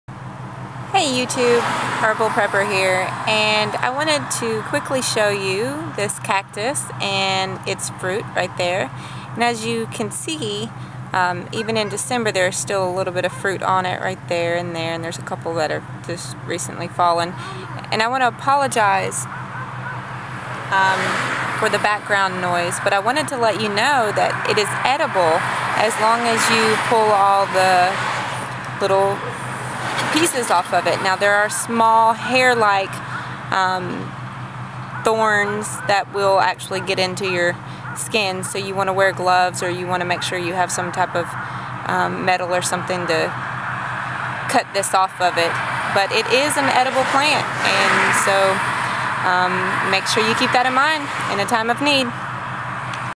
Foraging for prickly cactus
Keep in mind this WILL poke you, both the pads(flat parts) and the fruit(pear, purple colored) parts. Make sure you have gloves or something to hold onto in order to remove the needles safely(this will include the tiny hairlike ones as well) Like with almost all herbs and plants eating the young tender kind is much better than the older hard or fibrous leaves etc I stopped along the side of the road in someones yard to film this and therefore, I couldn't show you how to eat it bc the owner was already staring me down wondering what I was doing. LOL So here is a link on how to do that. Please make sure you cook it in order to cook out some of the alkaloids.